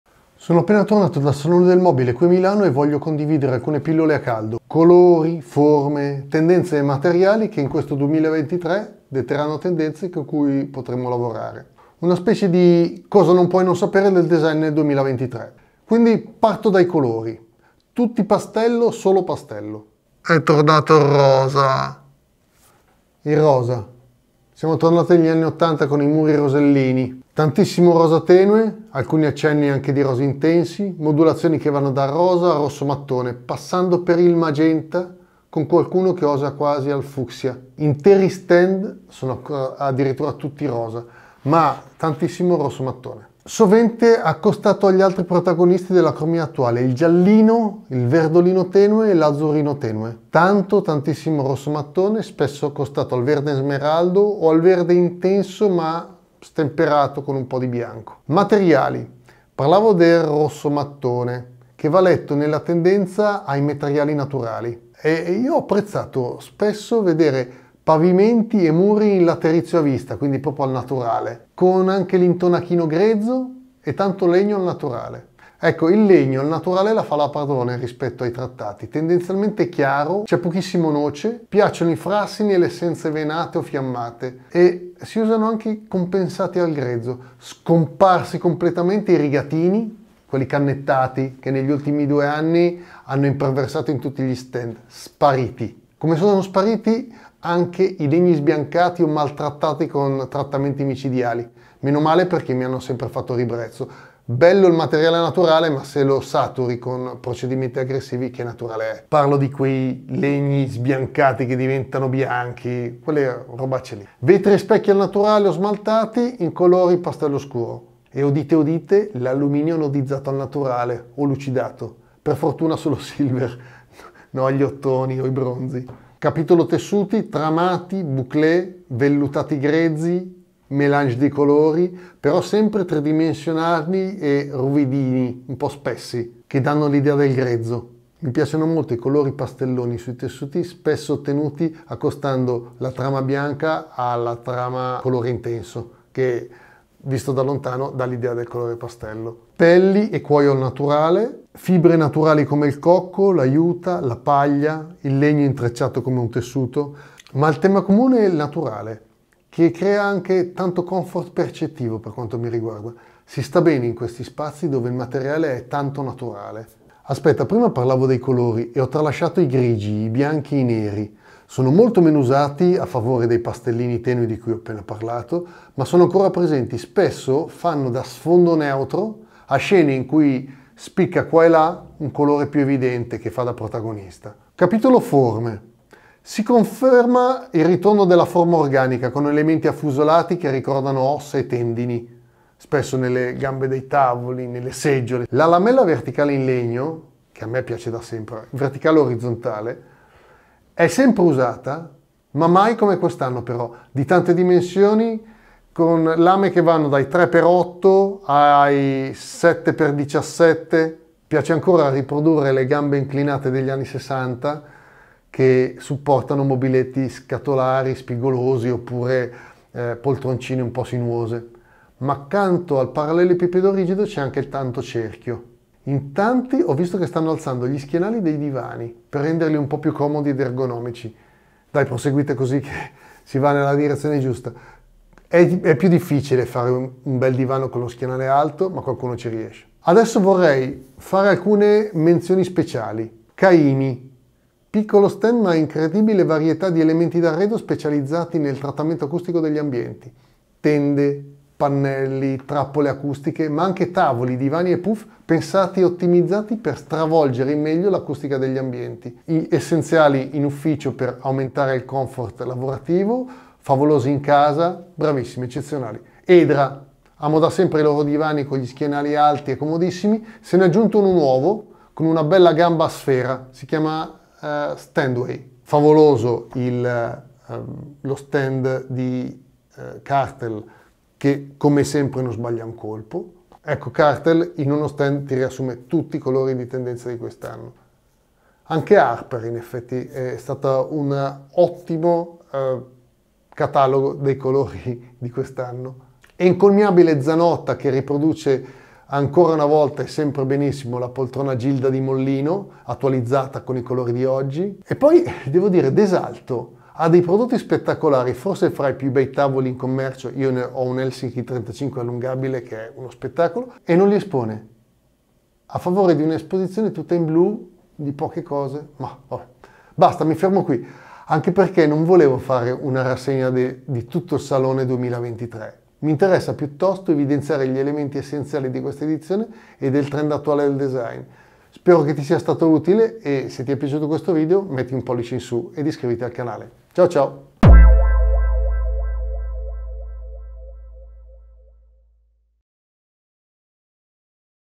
Salone del mobile 2023. Colori, forme e materiali da usare.
Intro  00:00
Colori 00:15
Materiali 00:56
Tessuti 02:05
Forme 03:03
Menzioni 04:00
Caimi 04:03
Edra 04:25
Kartell 04:34
Arper 04:47
Zanotta 04:55
Desalto  05:04
Finale  05:20